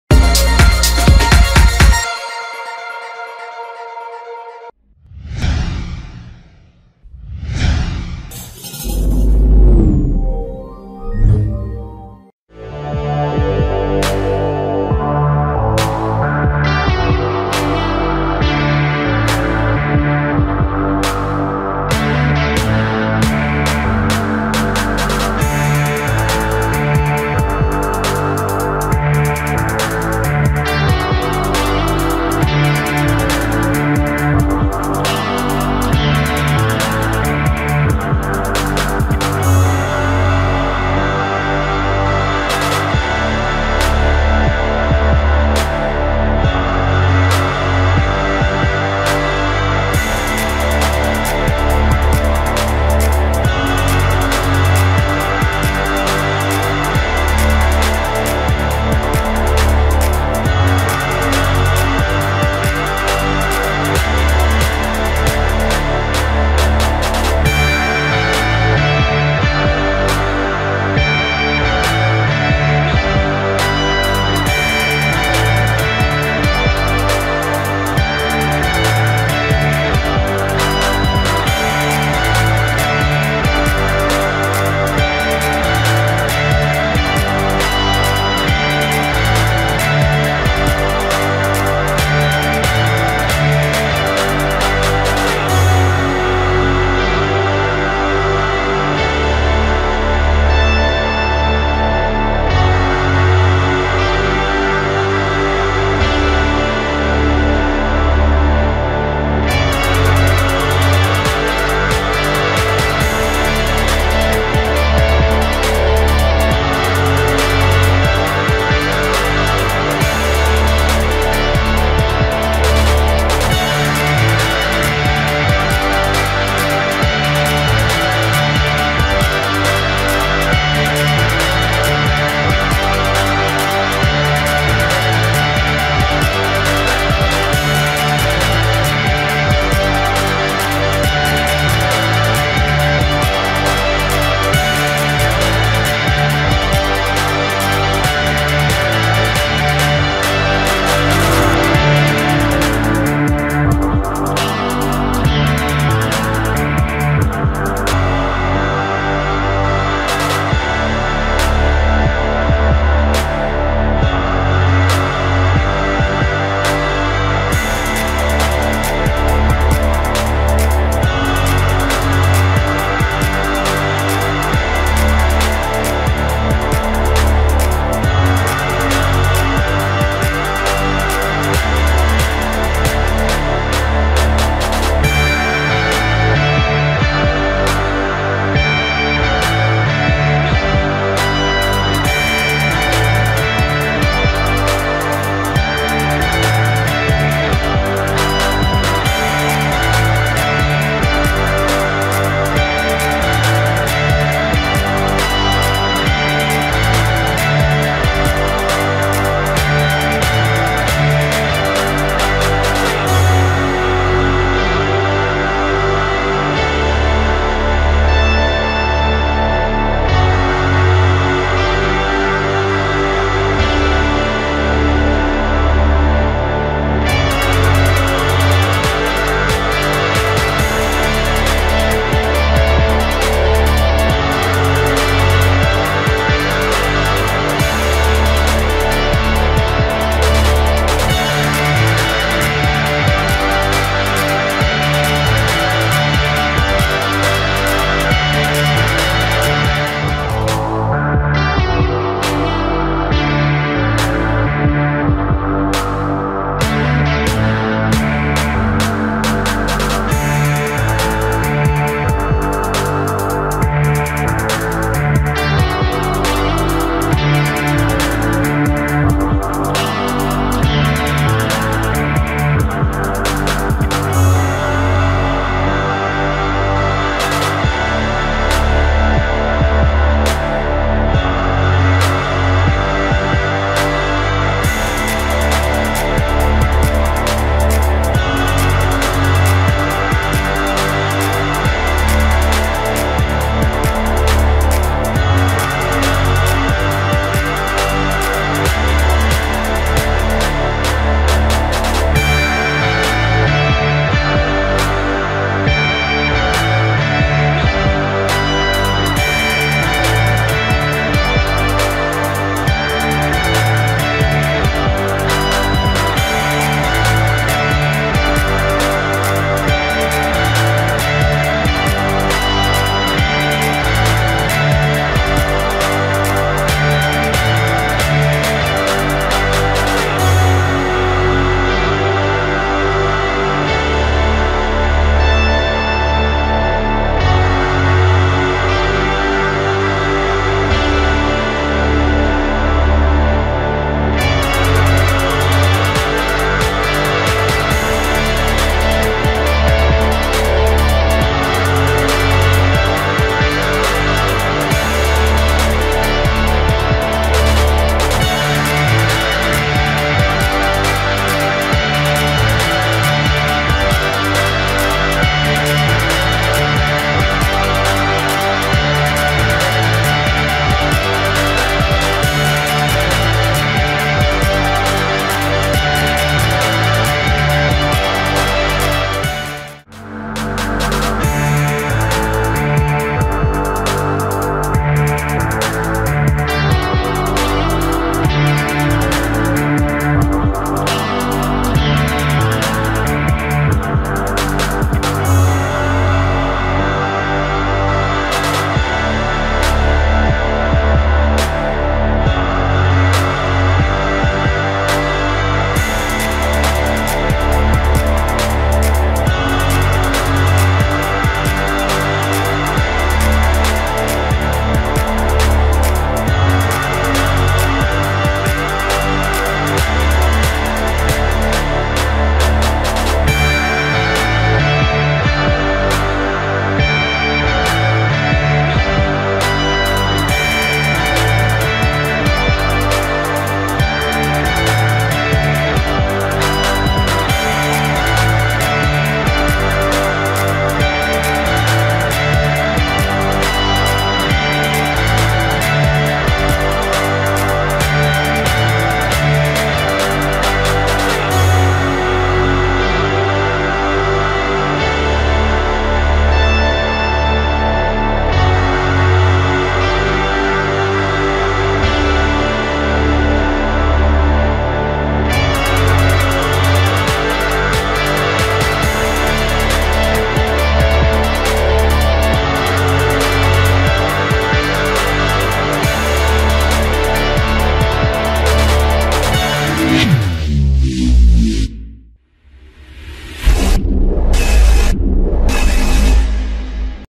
Phantom 4 Pro Plus over Water
Video with my DJI Phantom 4 Pro Plus on doing some quick maneuvers on and over the water.